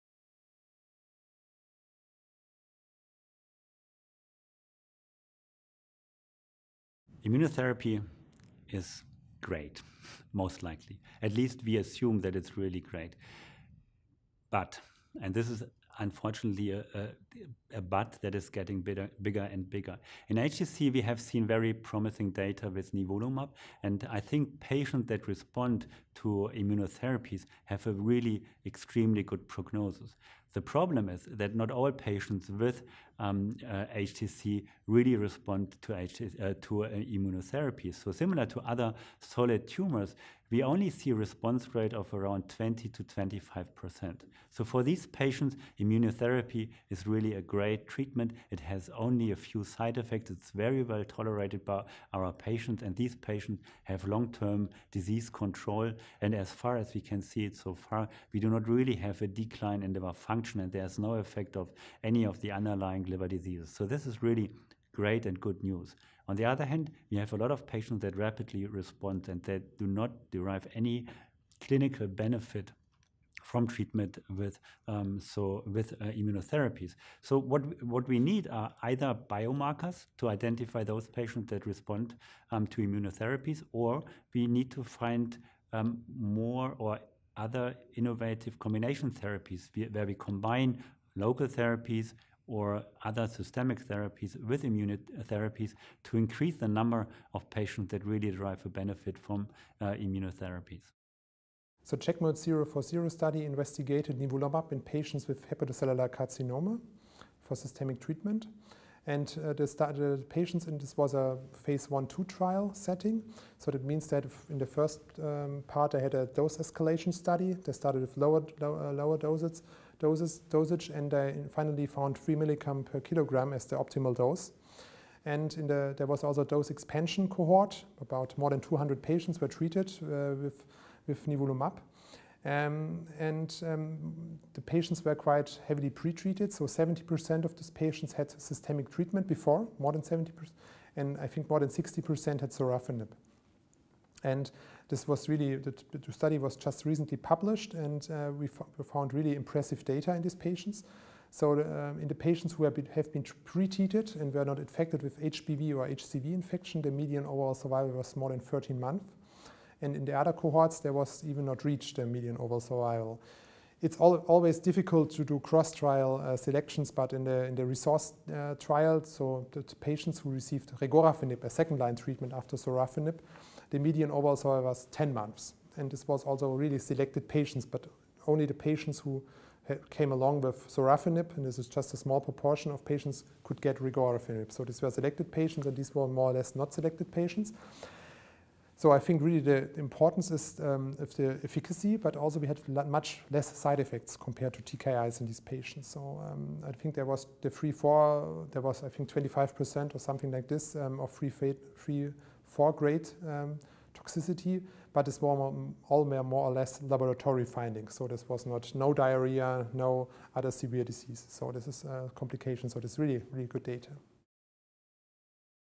Immunotherapy in Liver Cancer
Arndt Vogel, MD, and Oliver Waidmann, MD, discuss the potential for immunotherapy in liver cancer and review data from the CheckMate-040 clinical trial with nivolumab.